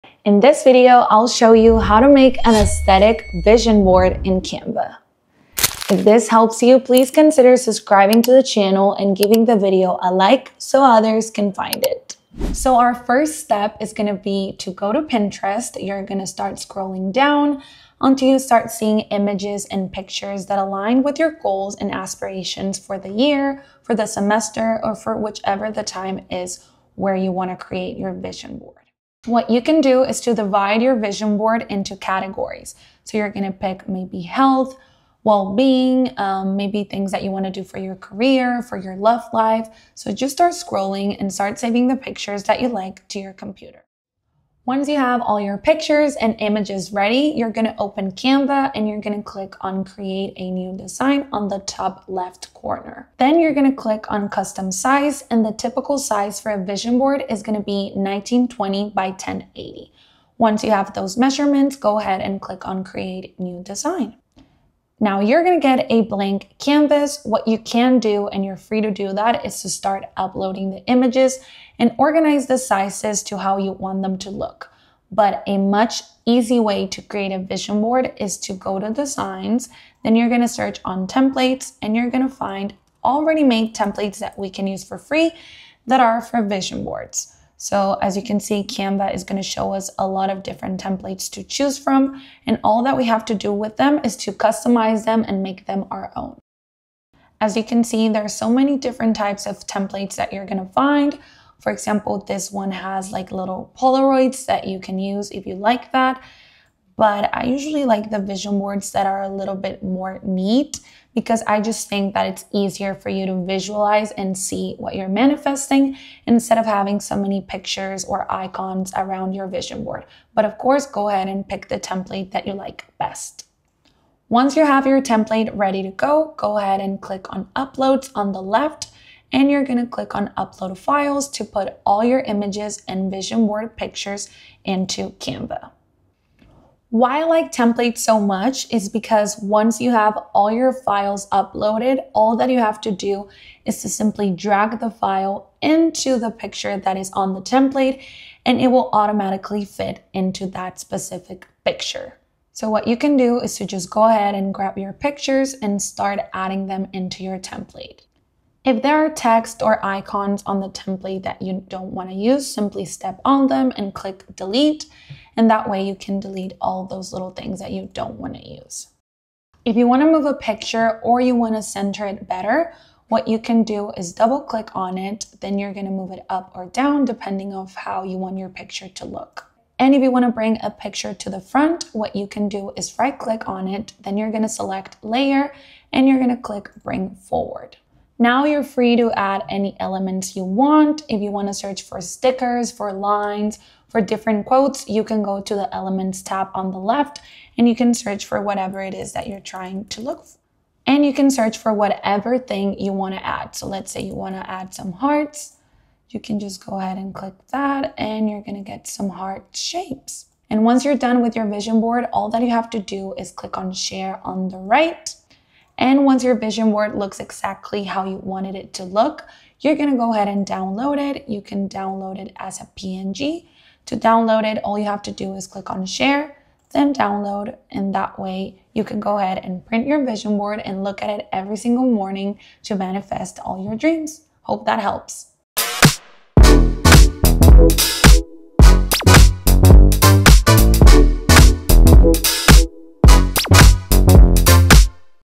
How to Make an Aesthetic Vision Board in Canva (Step-by-Step Tutorial!)
Start designing with Canva today. Use our link to explore tools, templates, and pro features:

In this video, I’ll go over how to make an aesthetic vision board in Canva to bring your dreams to life. You’ll learn how to choose the perfect design, add images that inspire you, and arrange everything in a visually pleasing way. Whether you want to manifest your goals or create a daily reminder of your aspirations, this step-by-step guide will help you design a vision board that reflects your ideal future.

Be sure to watch this video all the way as I cover essential tips like choosing the right background, using fonts that match your style, and adding motivational quotes. I’ll also show you how to layer elements, adjust transparency, and use Canva’s built-in tools to make your board look polished and professional. By the end, you’ll have a beautiful, organized vision board that keeps you focused on your goals.

Creating a vision board is a great way to visualize success and stay motivated. With Canva’s easy-to-use design features, you can make a digital or printable vision board that matches your aesthetic. Whether you're setting goals for 2025, planning your dream life, or just want a creative way to stay inspired, this tutorial will help you bring your vision to reality.

GuideWise is here to help you learn new skills, solve problems, and make life easier.

– Mari

Timeline:
0:00 - Introduction
0:12 - Pinterest
0:28 - Dividing Your Vision Board
0:44 - Canva
0:49 - Creating a New Design
1:18 - Template Search
1:37 - Selecting a Template
2:03 - Uploading Files
2:23 - Editing The Vision Board
3:31 - Sharing The Vision Board
3:40 - Downloading The Vision Board